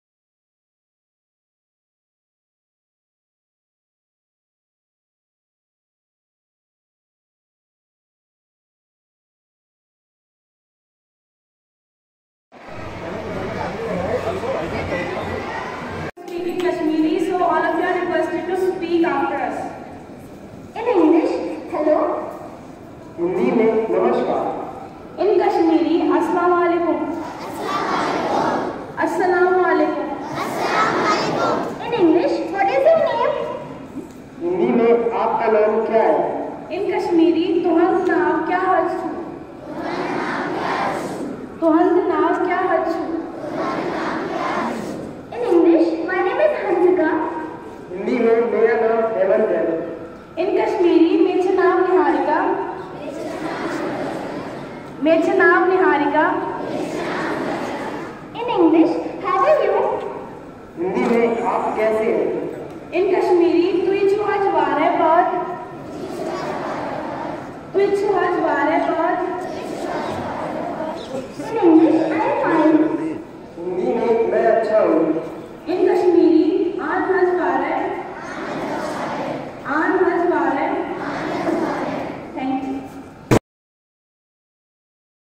KV3 Gwalior Kashmiri
Language: KASHMIRI
Name of the School: K. V. No.3, Gwalior
Address: Morar Cantt, Gwalior 474006
State: Madhya Pradesh
Detail of Video: Conversation in Kashmiri Language performed in morning assembly